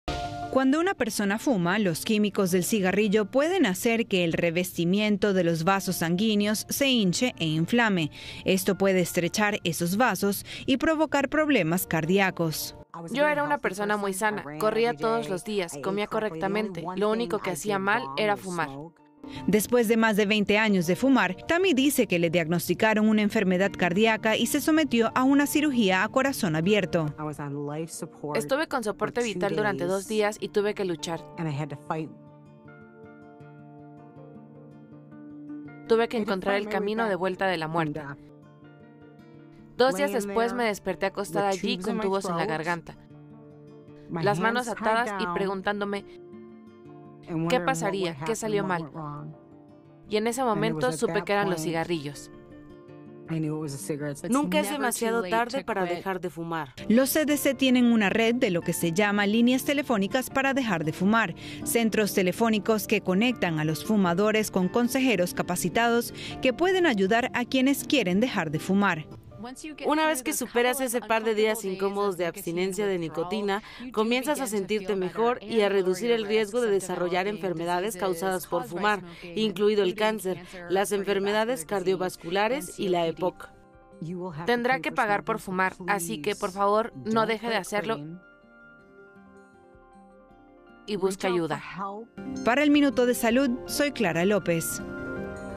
FUMAR M4TA | Tammy fumó 20 años y terminó sometida a una cirugía a corazón abierto
FUMAR MATA

MinutoDeSalud | Tammy es un caso más de que el uso de cigarrillo desencadena una serie de complicaciones en la salud. Tras fumar 20 años, fue sometida a una cirugía a corazón abierto.

Nota de Clara López.